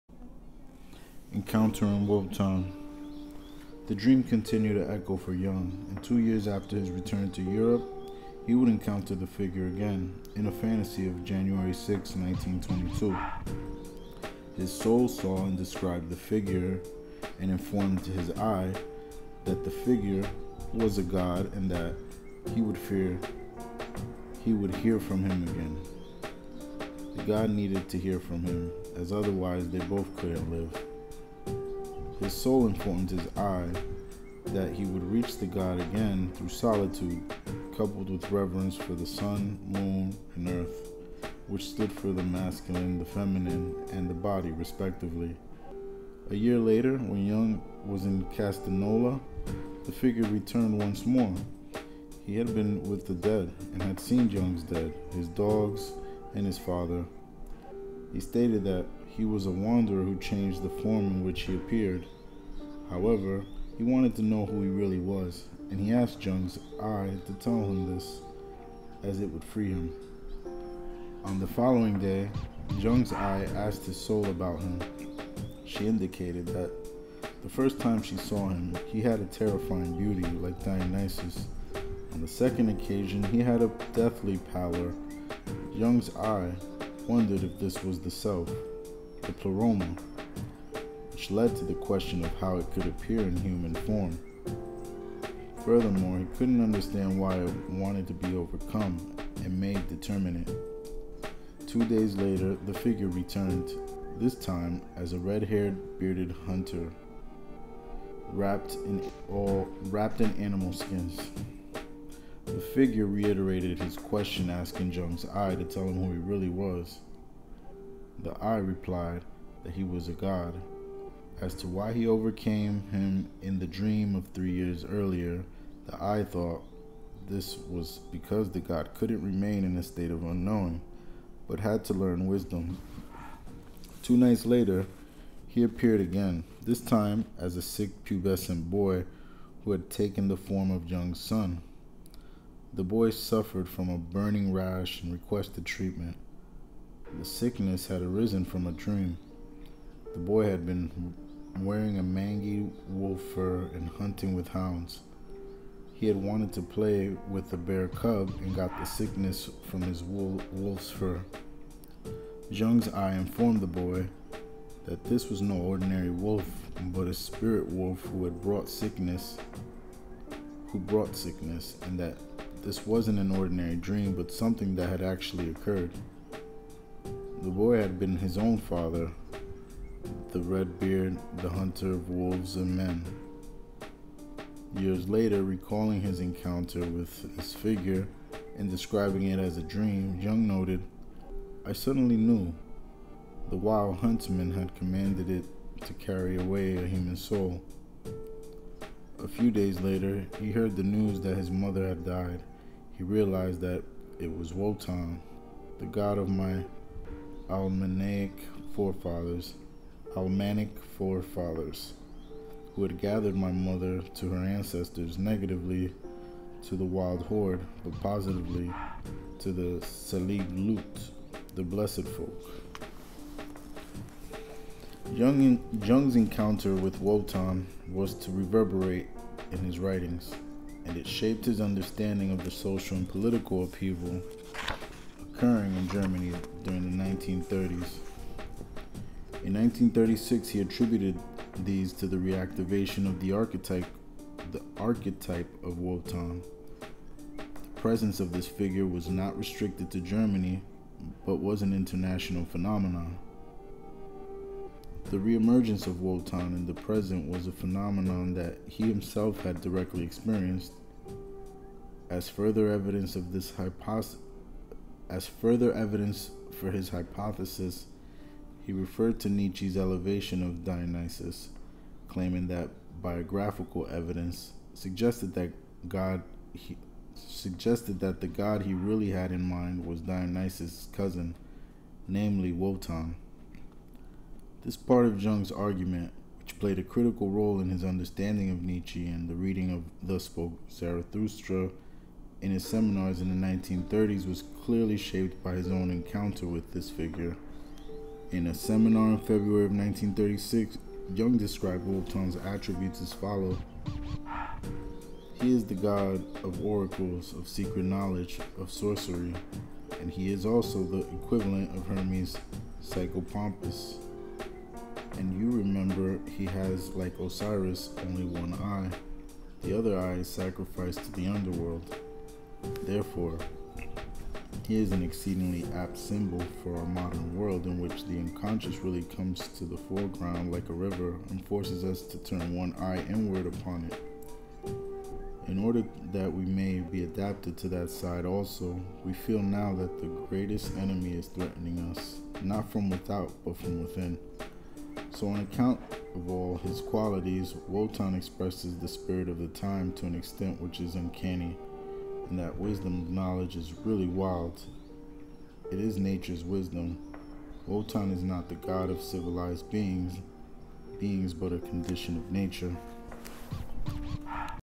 Encountering Wotan (16 of 25) The Black Books Vol1
Expresses the spirit of the times, natures wisdom, not the god of civilized being but a condition of nature

Wotan has 1 eye, other eye sacrificed to the underworld

Wants to be overcome and made determinate